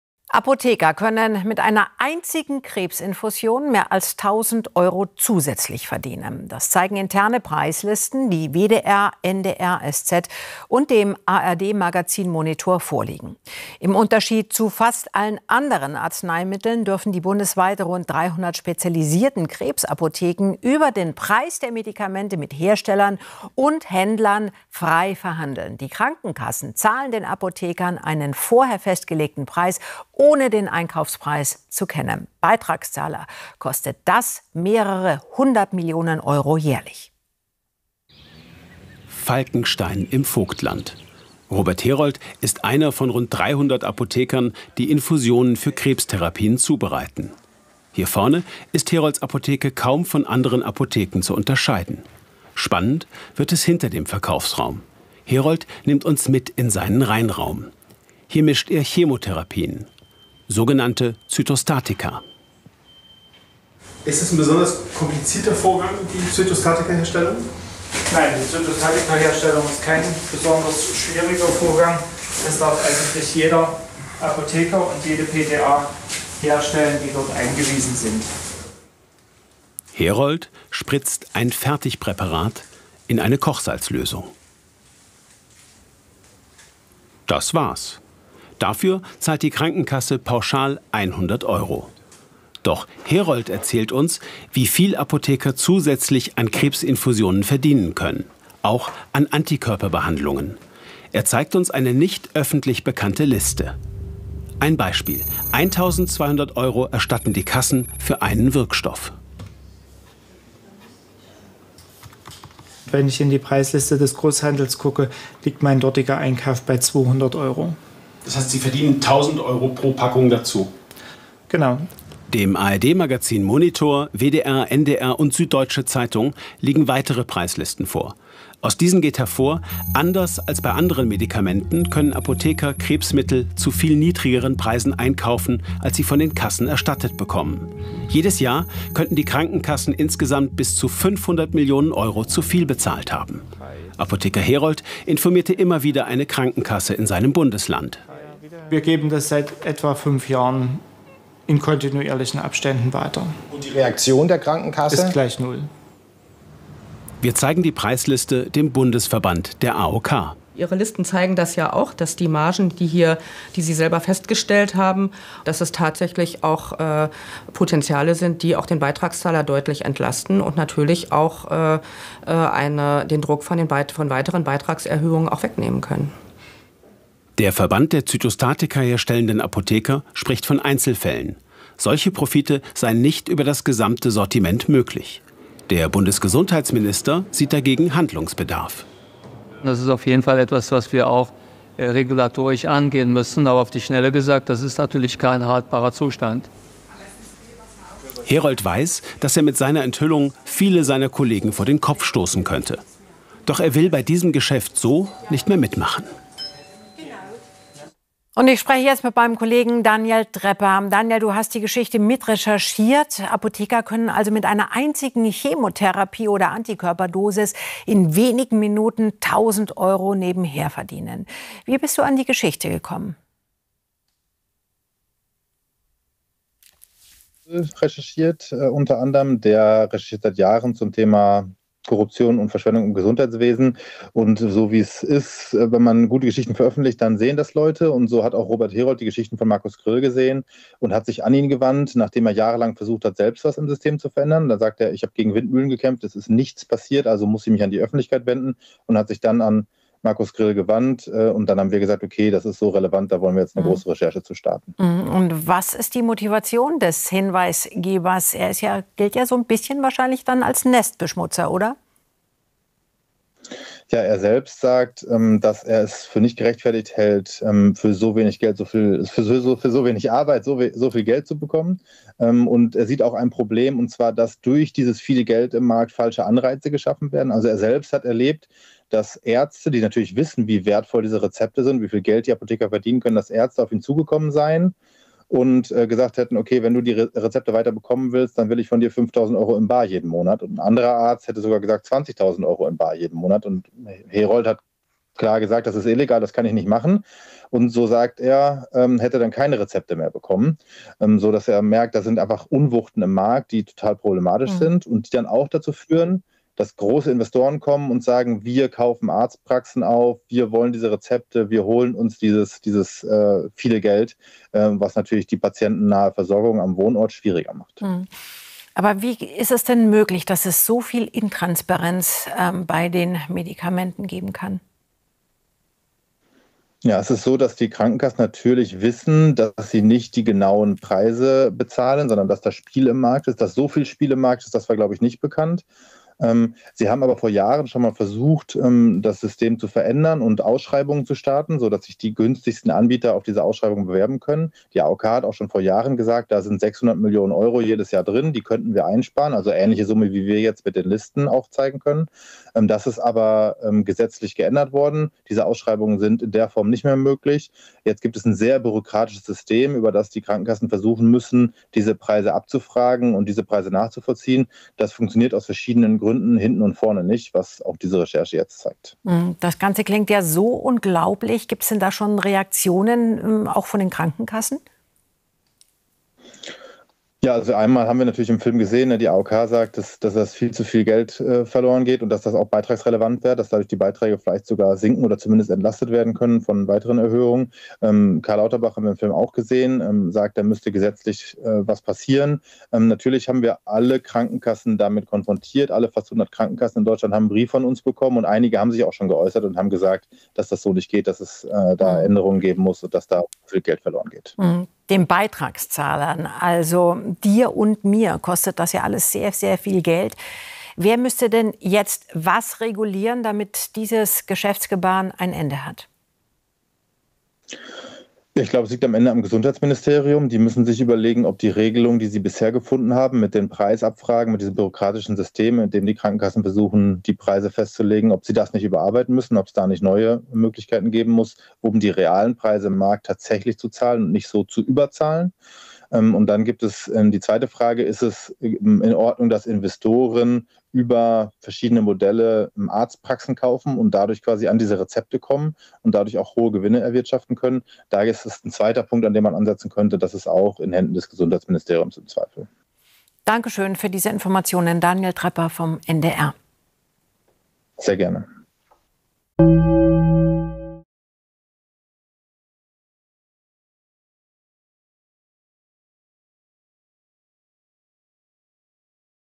Apotheker als Whistleblower: Milliarden-Verschwendung bei Krebsmedikamenten
Apotheker, die Krebsmedikamente herstellen, können mit einer einzigen Infusion mehr als 1000 Euro extra verdienen. Das zeigen interne Preislisten, die WDR, NDR, SZ und Monitor vorliegen. Die Krankenkassen könnte das jährlich bis zu 500 Millionen Euro kosten.